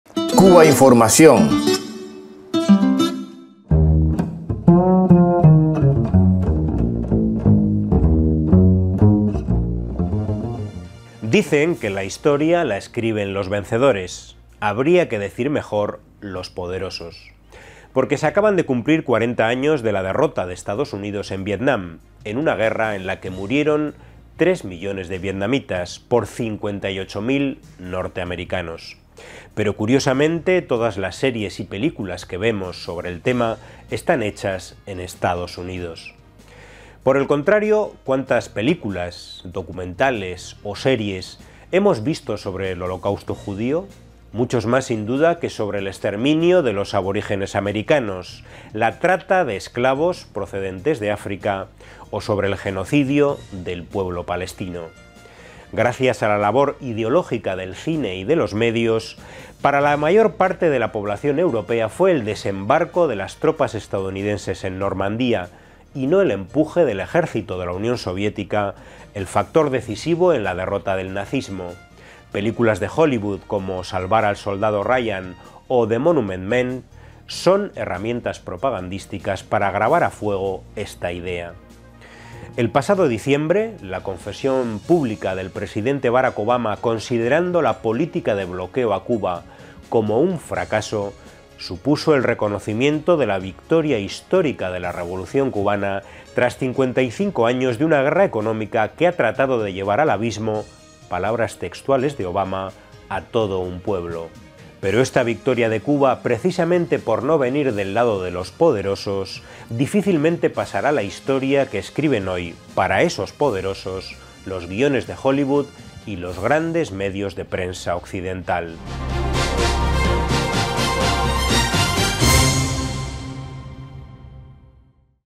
Vietnam y Cuba vencieron a EEUU. Pero a veces la Historia la escriben los perdedores
Cubainformación TV – Basado en un texto de Iroel Sánchez – Blog “La pupila insomne”.- Dicen que la historia la escriben los vencedores. Habría que decir mejor... los poderosos. Porque se acaban de cumplir 40 años de la derrota de EEUU en Vietnam, en una guerra en la que murieron 3 millones de vietnamitas, por 58.000 norteamericanos. Pero –curiosamente- todas las series y películas que vemos sobre el tema están hechas en EEUU. Edición: José Ignacio Antón.